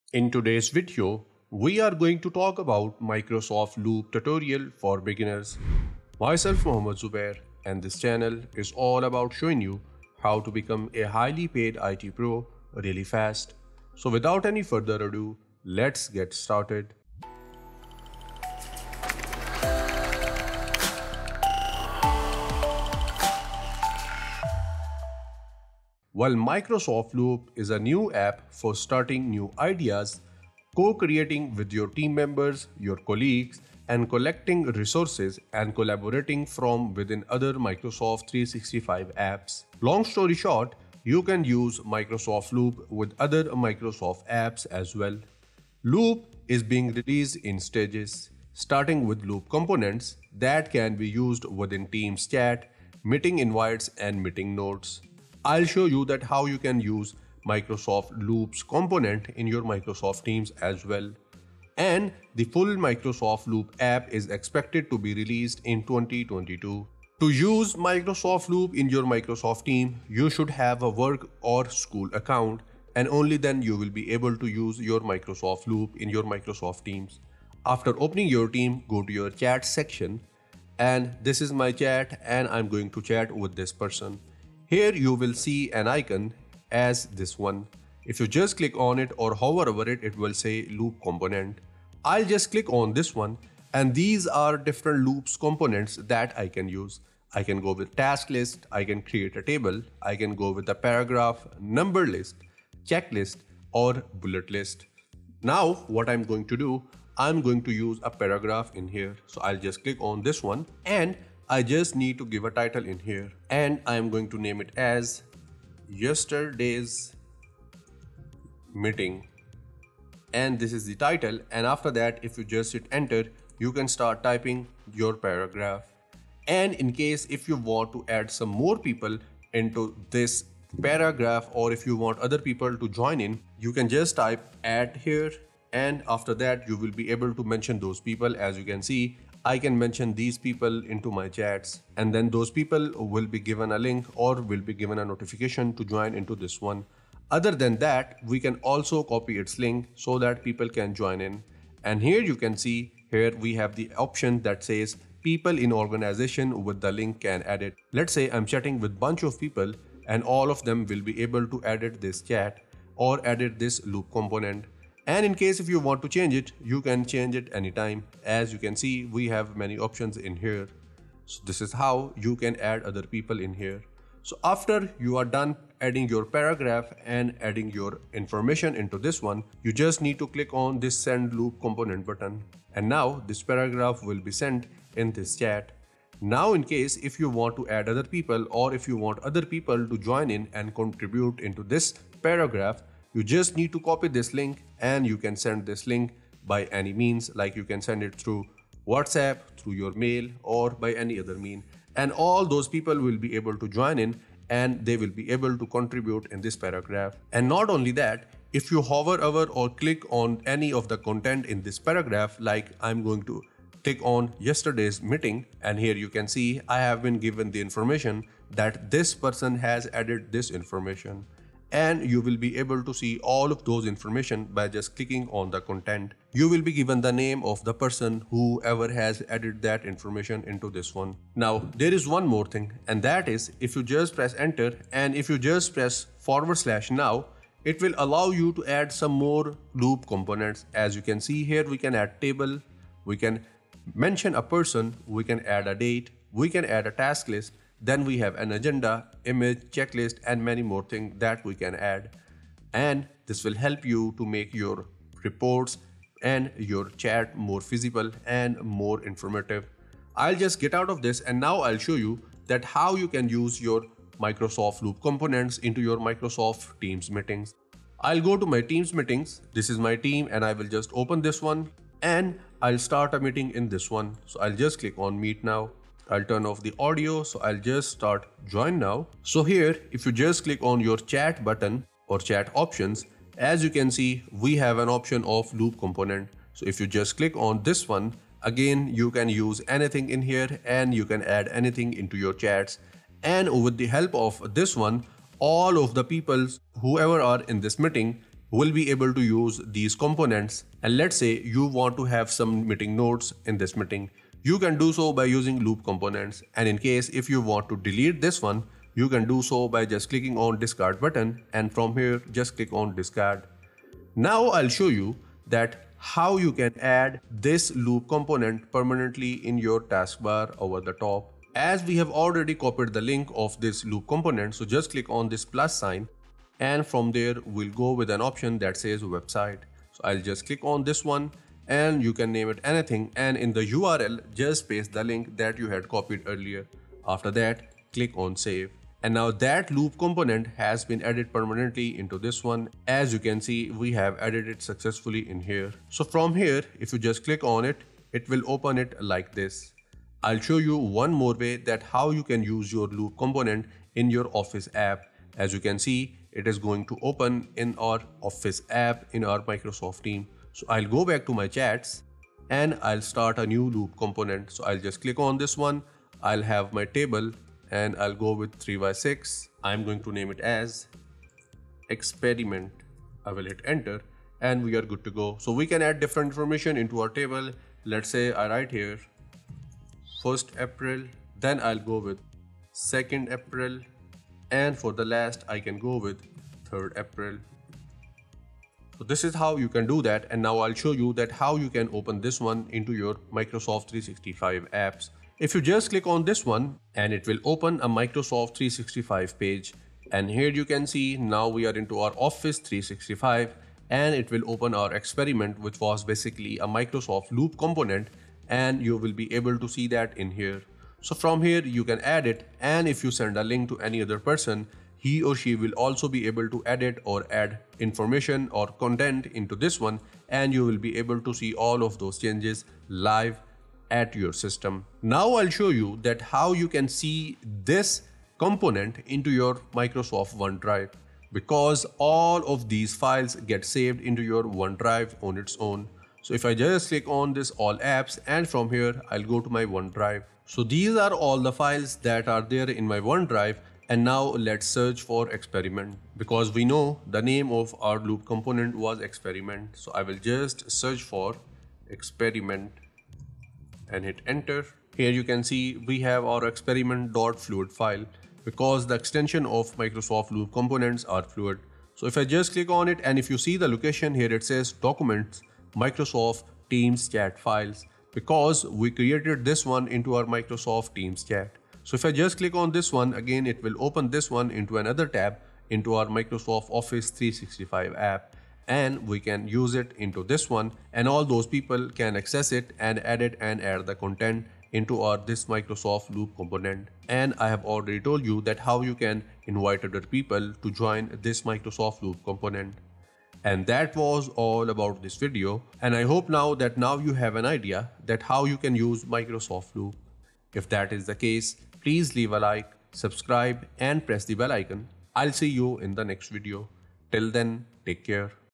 Discover Microsoft Loop | Tutorial for Beginners
Microsoft Loop is an app that combines a powerful and flexible canvas with portable components that stay in sync and move freely across Microsoft 365 apps.

In this video, I will walk you through what Microsoft Loop is.

⏰Timestamps⏰

00:00 Intro
01:07 How to Use Microsoft Loop
03:05 Add or Invite People to Loop Component
03:54 Additional Loop Component
04:21 Loop in Teams Meetings
05:14 Add Loop on TaskBar in Teams
07:04 Loop Component in One Drive
08:13 Outro

💻 To get more of our best content on IT careers and IT certifications, go